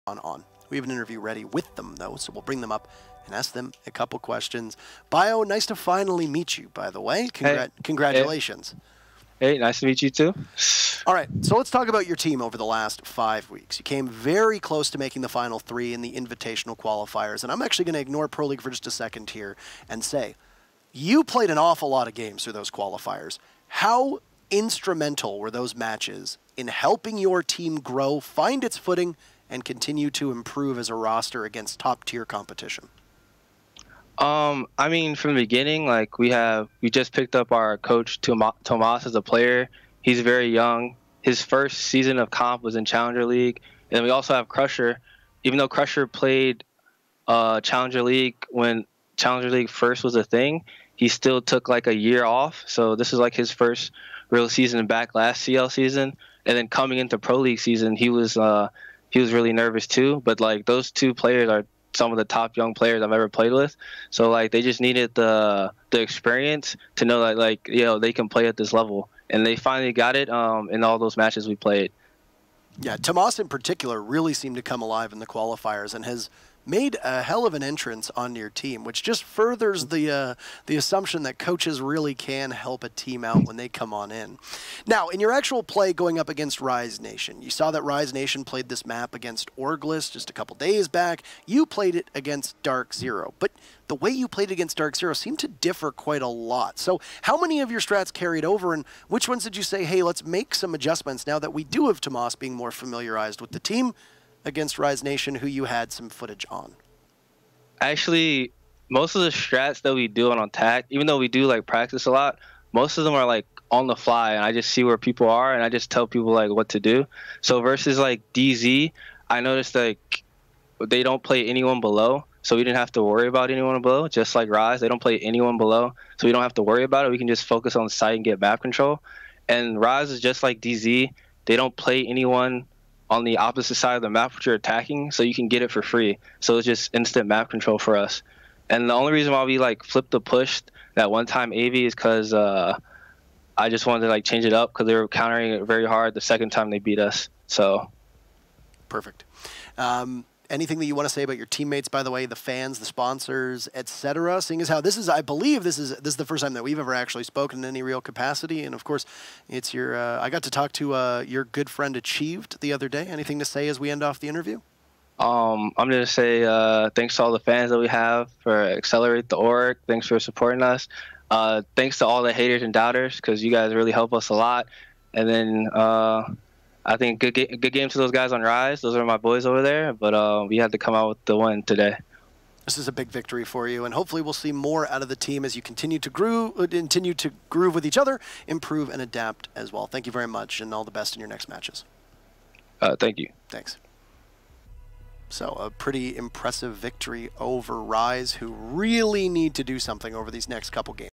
Excelerate vs. Rise post interview (biologic)
biologic's post interview after Excelerate Gaming won against Rise Nation during Rainbow Six Pro League Season 9 NA matchday 5